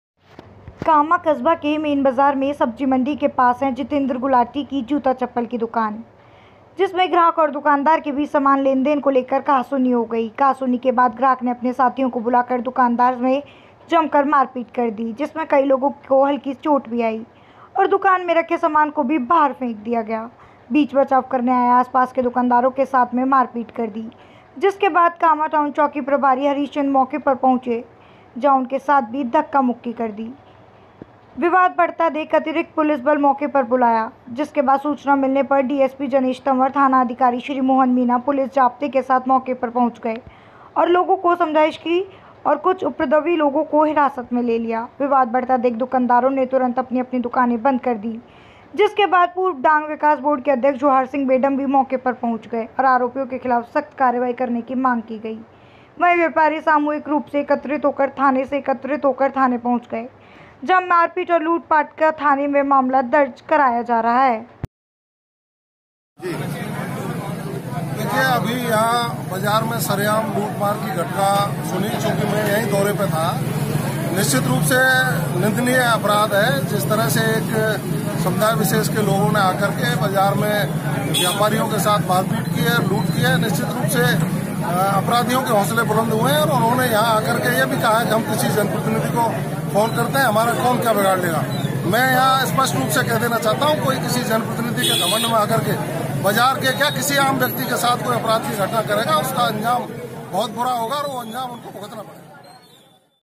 Kaamaa दबंगों ने की दुकानदारों से मारपीट
कामांं कस्बा के मेन बाजार में सब्जी मंडी के पास है जितेंद्र गुलाटी की जूता चप्पल की दुकान में ग्राहक और दुकानदार के बीच सामान लेनदेन को लेकर कहासुनी हो गई कहासुनी के बाद ग्राहक ने अपने साथियों को बुला कर दुकानदार से जमकर मारपीट कर दी जिसमें कई लोगों की हल्की चोट भी आई है और  दुकान में रखे सामान को भी बाहर फेंक दिया बीच-बचाव करने आए आसपास के दुकानदारों के साथ में मारपीट कर दी जिसके बाद कामां टाउन चौकी प्रभारी हरिश्चंद्र मौके पर पहुंचे जहां उनके साथ भी धक्का-मुक्की कर दी गई कर दी गई विवाद बढ़ता देख अतिरिक्त पुलिस बल मौके पर बुलाया जिसके बाद सूचना मिलने पर डीएसपी जनेश तवंर ,थानाधिकारी श्री मोहन मीणा पुलिस जाब्ते के साथ मौके पर पहुंच गए और लोगों से समझाइश की और कुछ उपद्रवी लोगों को हिरासत में ले लिया विवाद बढ़ता देख दुकानदारों ने तुरंत अपनी अपनी दुकानें बंद कर दी जिसके बाद पूर्व डांग विकास बोर्ड के अध्यक्ष जवाहर सिंह बेढम भी मौके पर पहुंच गए और आरोपियों के खिलाफ सख्त कार्रवाई करने की मांग की गई वहीं व्यापारी सामूहिक रूप से एकत्रित होकर थाने से एकत्रित होकर थाने पहुंच गए जहां मारपीट और लूटपाट का थाने में मामला दर्ज कराया जा रहा है।

जवाहर सिंह बेढम पूर्व मंत्री।